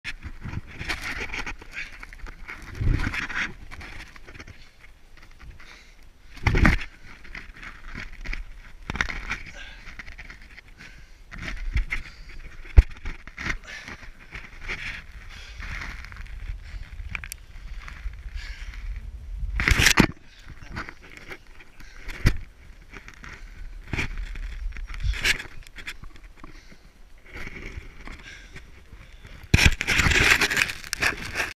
Gopro, caving in Washington
Coming out of a squeeze spot